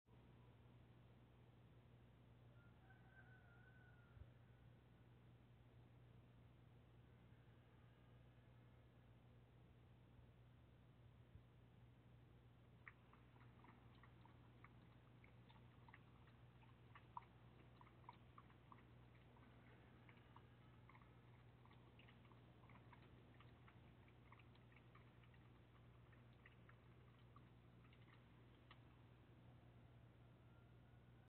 coyote gets drink
coyote comes to the water dishes